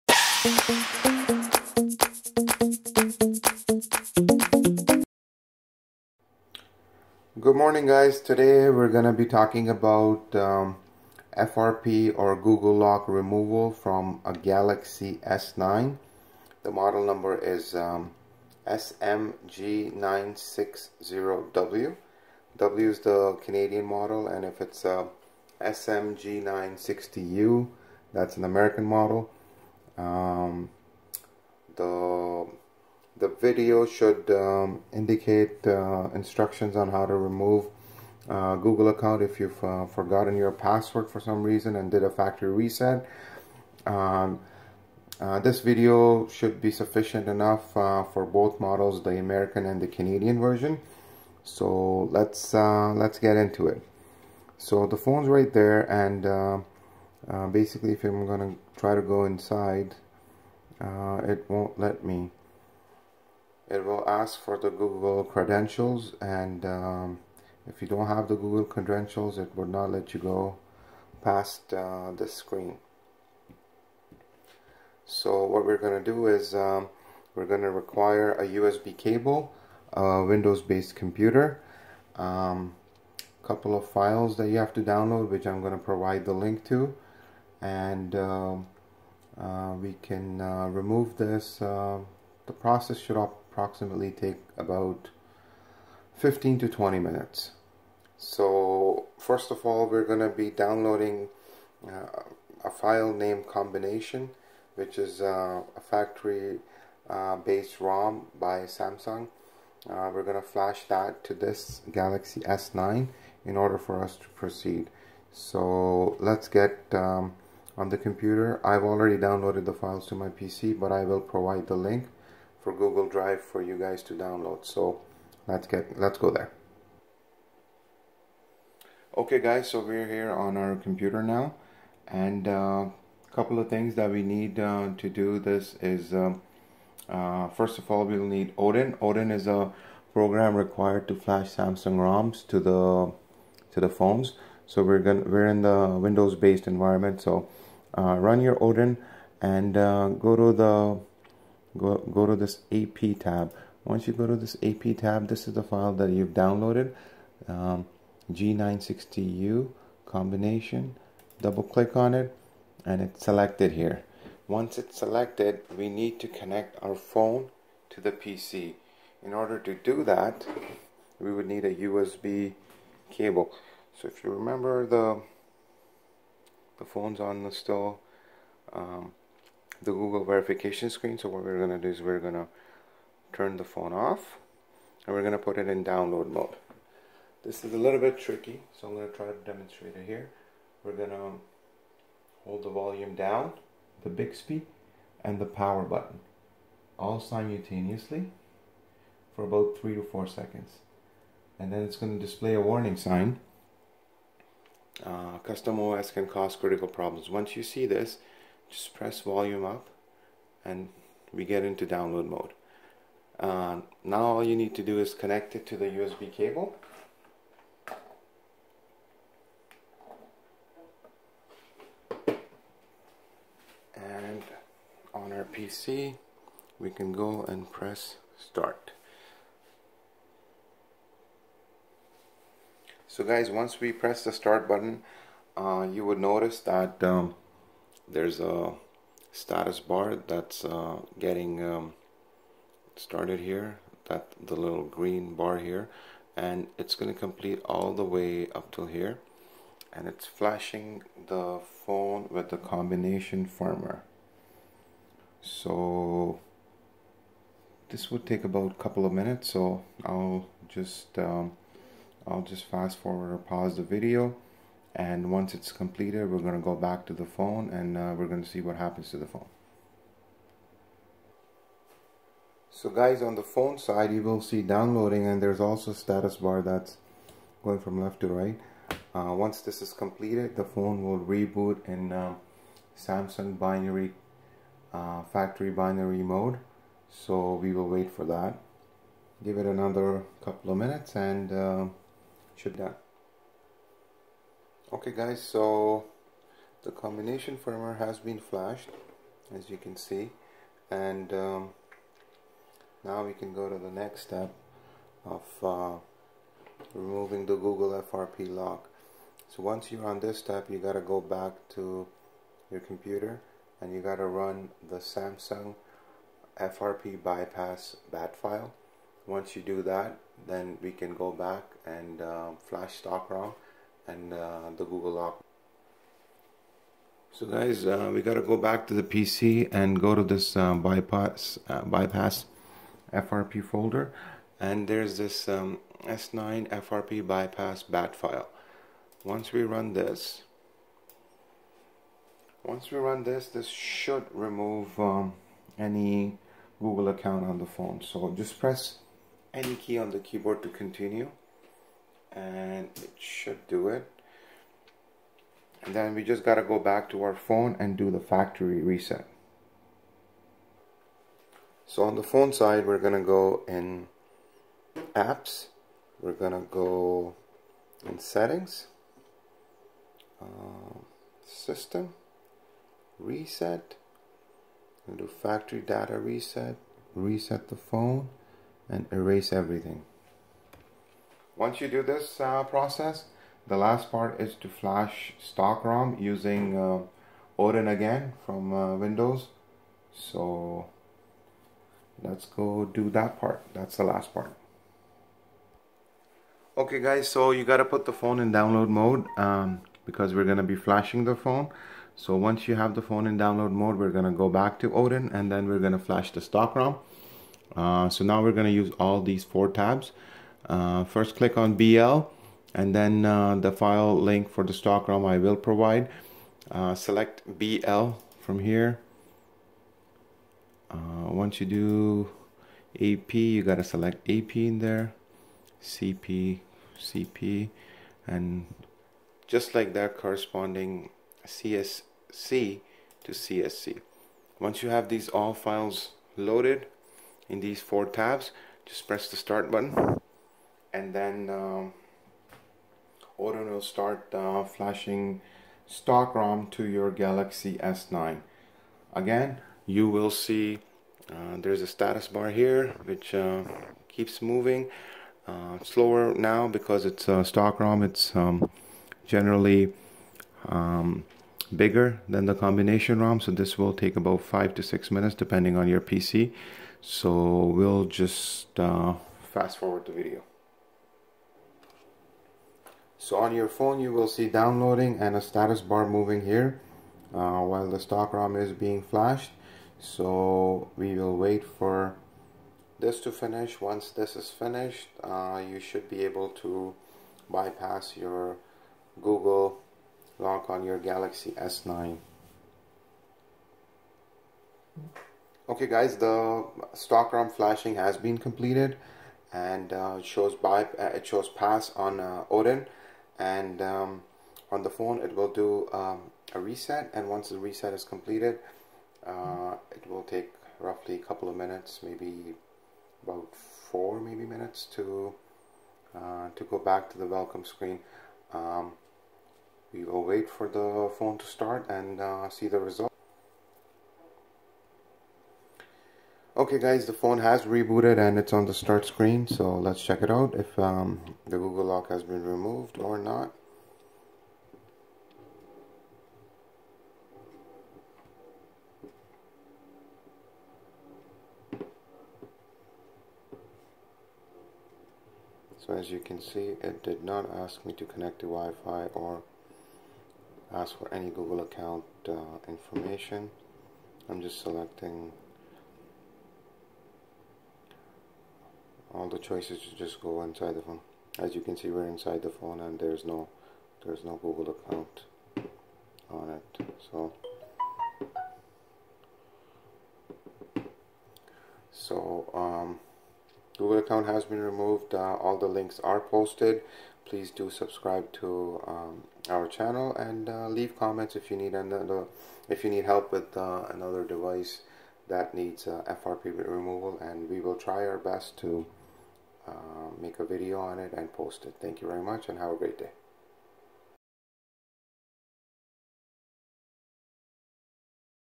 Galaxy S9 SM-G960 FRP Google Samsung Account removal
This video will illustrate a Galaxy S9 SM-G960 which is google locked upon a factory reset. We will provide step by step on how to remove the google account on this device permanently.

For any reason you find this process too hard, or are unable to do it we do offer remote google account removal services. (PAID SERVICE)